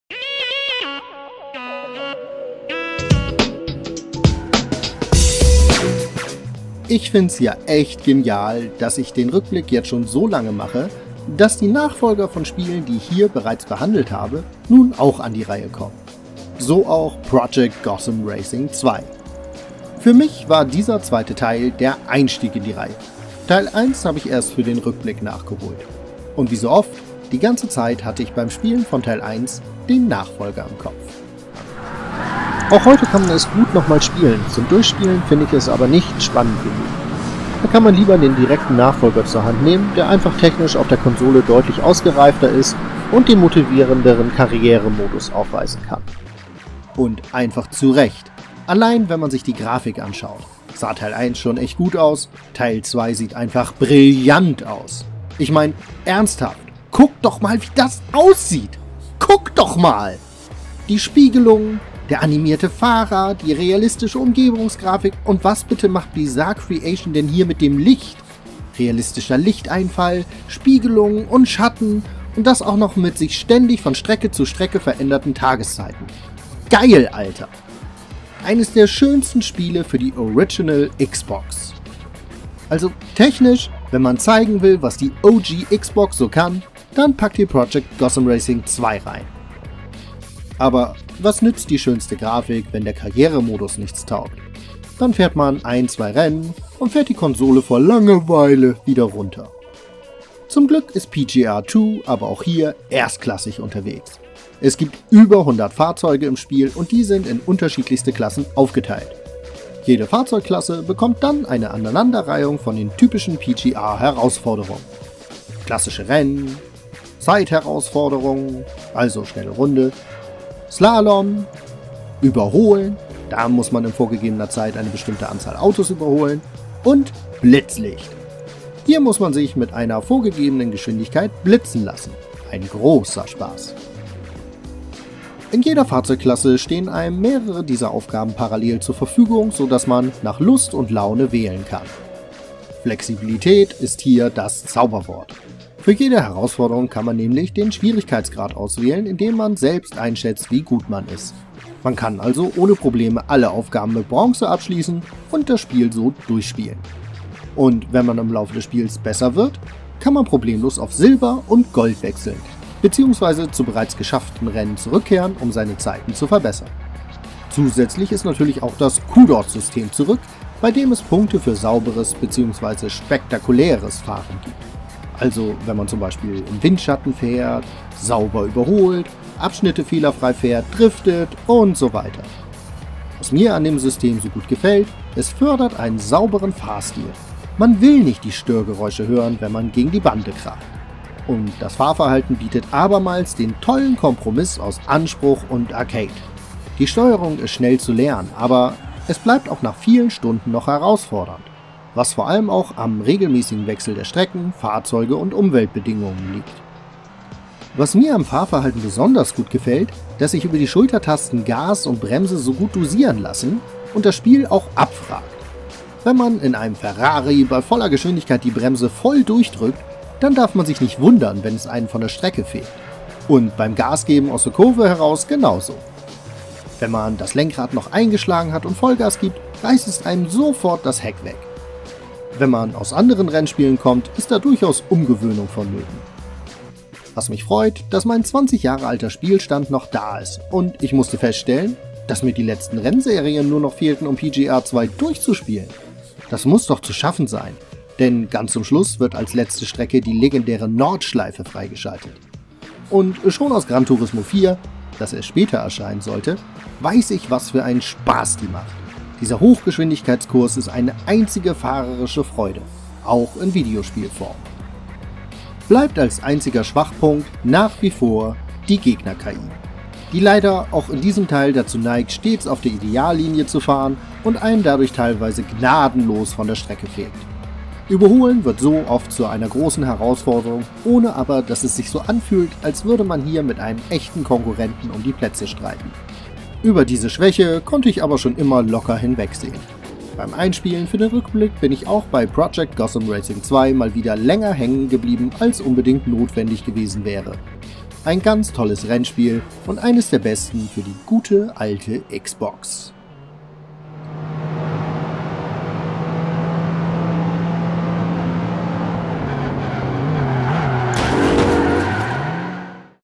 Project Gotham Racing 2 - Retro Spiele Rückblick
Project Gotham Racing 2 ist der in allen belangen verbesserte Nachfolger und gehört zu den besten Rennspielen, die man auf der Original Xbox spielen kann. Ich habe etliche Stunden ins Spiel investiert und kann es auch heute noch Retro Gamern bedenkenlos ans Herz legen.